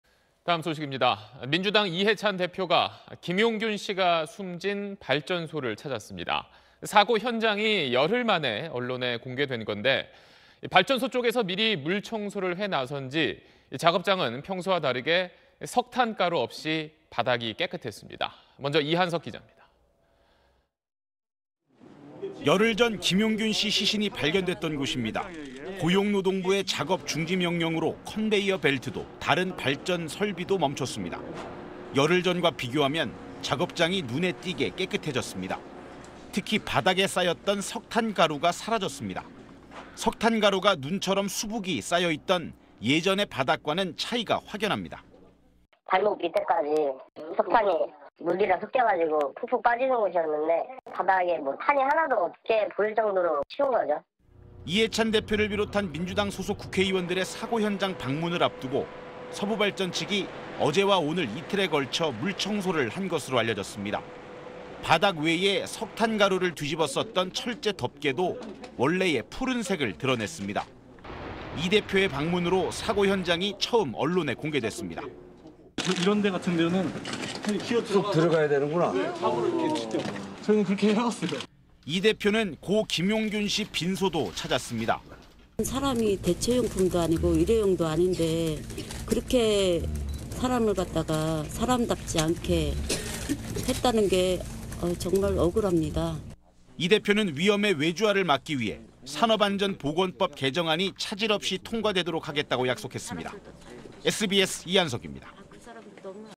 "여당 대표 온다고 물청소"…석탄가루 사라진 발전소 / SBS
민주당 이해찬 대표가 김용균 씨가 숨진 발전소를 찾았습니다. 사고 현장이 열흘 만에 언론에 공개된 건데 발전소 쪽에서 미리 물청소를 해놔서인지 작업장은 평소와 다르게 석탄 가루 없이 바닥이 깨끗했습니다.
먼저 이한석 기자입니다.